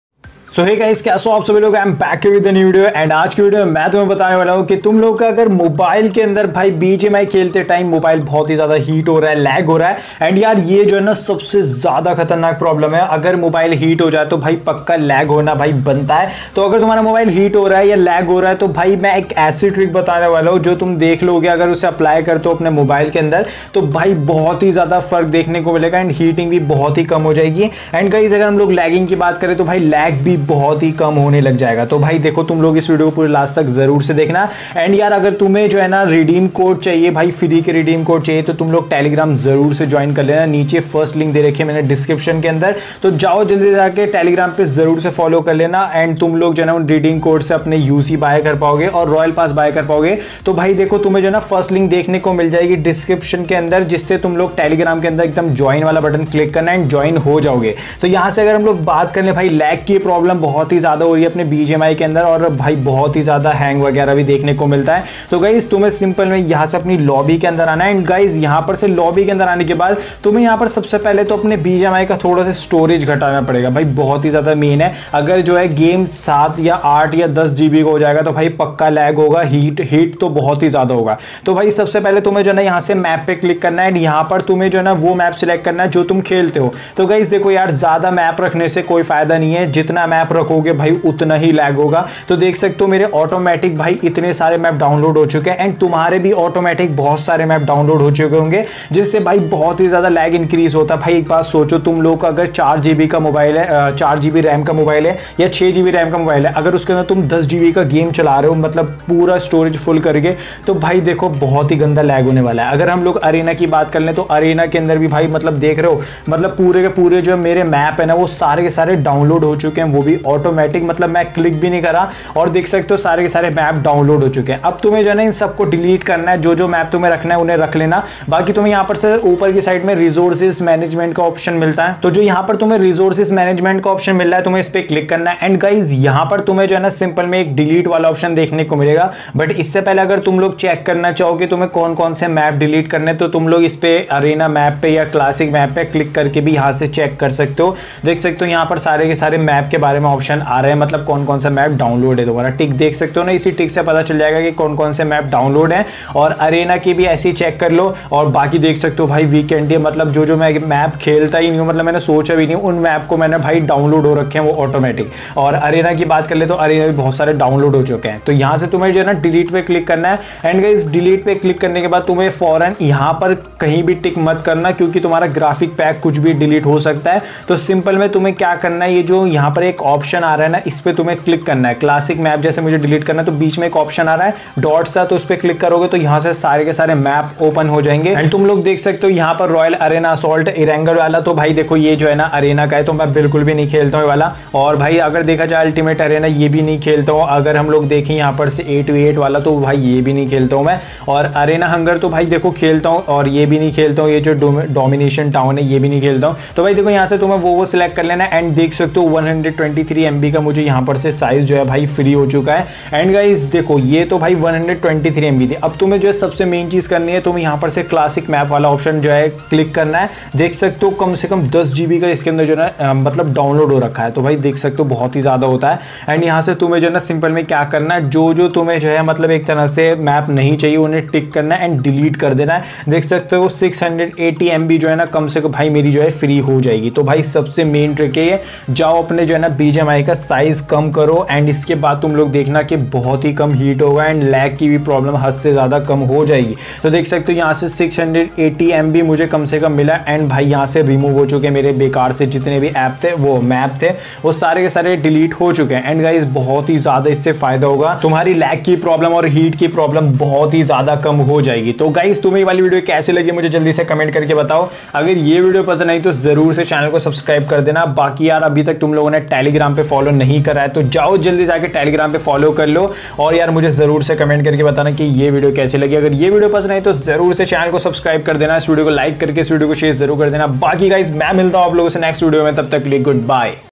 BGMI Mobile Heating & Lag Issue Permanently Fix | How to Fix BGMI Overheating Problem in Android
MUSIC NAME - NCS

Video Cover ⬇️⬇️

bgmi 2.5 lag fix setting
how to fix lag in bgmi in 2gb ram